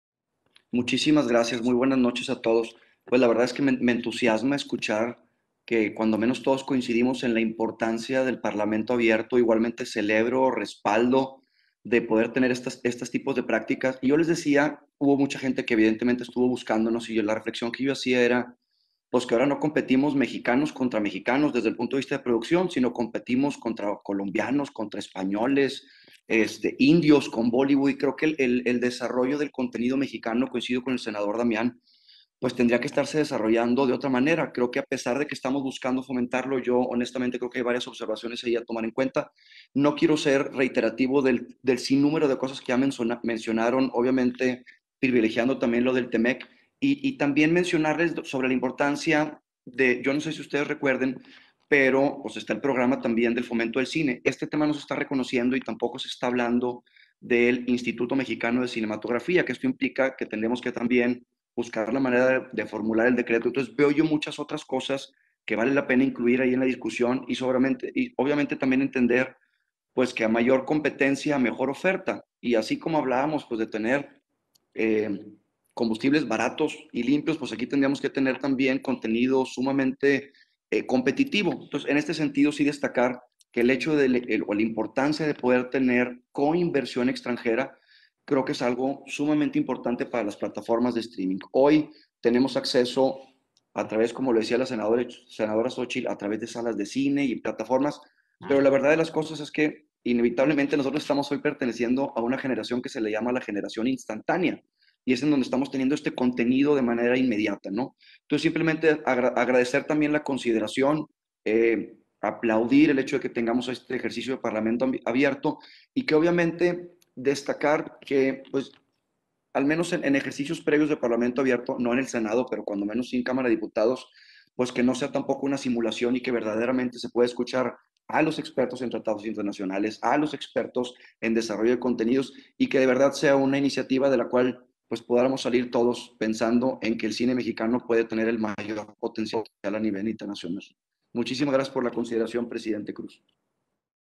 Senador Horacio González, en reunión extraordinaria de comisiones unidas de Radio Televisión
26 de abril de 2021.- Intervención del senador Jesús Horacio González Delgadillo durante la reunión extraordinaria de las comisiones unidas de Radio Televisión y Cinematografía, y de Estudios Legislativos, Segunda, para referirse al dictamen con Proyecto de Decreto por el que se adiciona una fracción X al artículo 223 de la Ley Federal de Telecomunicaciones y Radiodifusión.